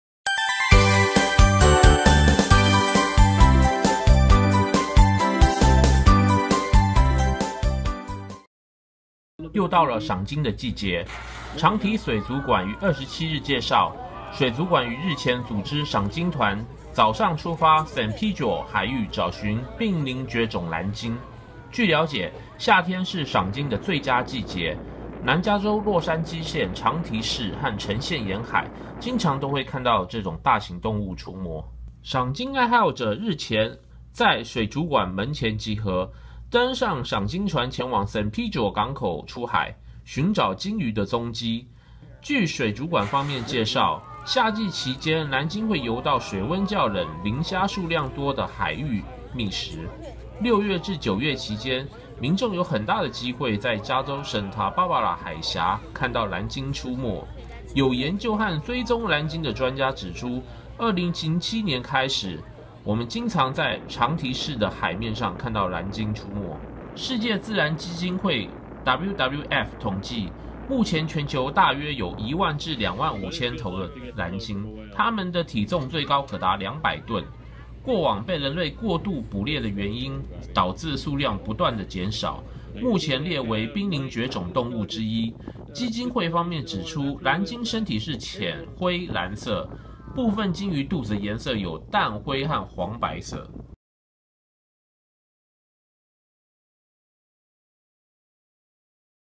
賞鯨季節 找尋瀕臨絕種藍鯨
影片由長堤水族館提供

長堤水族館於27日介紹，水族館於日前組織賞鯨團早上出發聖佩卓(San Pedro)海域找尋瀕臨絕種藍鯨。據了解，夏天是賞鯨的最佳季節，南加州洛杉磯縣長堤市(Long Beach)和橙縣沿海經常都會看到動物出沒。

眾賞鯨愛好者於26日早上在水族館門前集合，登上賞鯨船前往聖佩卓港口對出海面，尋找鯨魚蹤影。根據水族館介紹，夏季期間藍鯨游到水溫較冷、而且磷蝦數量多的的水域覓食。在6月至9月期間，民眾很大機會在加州聖他芭芭拉海峽(Santa Barbara Channel)附近看到藍鯨出沒。有研究和追蹤藍鯨的專家指出，藍鯨自2007年開始經驗在長堤市對出的海面活動。

根據世界自然基金會(WWF)統計，目前全球大約有1萬至2萬5000隻，牠們的體重最高可達200噸重。由於過往被人類過度捕獵的原因導致數量不斷減少，目前被列為瀕臨絕種動物之一。基金會指出，藍鯨的身體是淡灰藍色，部分鯨魚肚子的顏色有淡灰或黃白色。